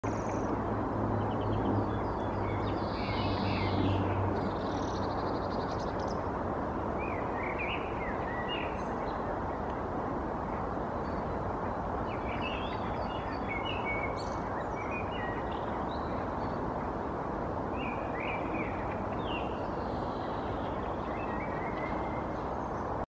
Greenfinch singing in the tree (take #2)
European Greenfinch (Carduelis chloris) perching in the tree, singing.

Viherpeippo (Carduelis chloris) istuu puussa ja laulaa.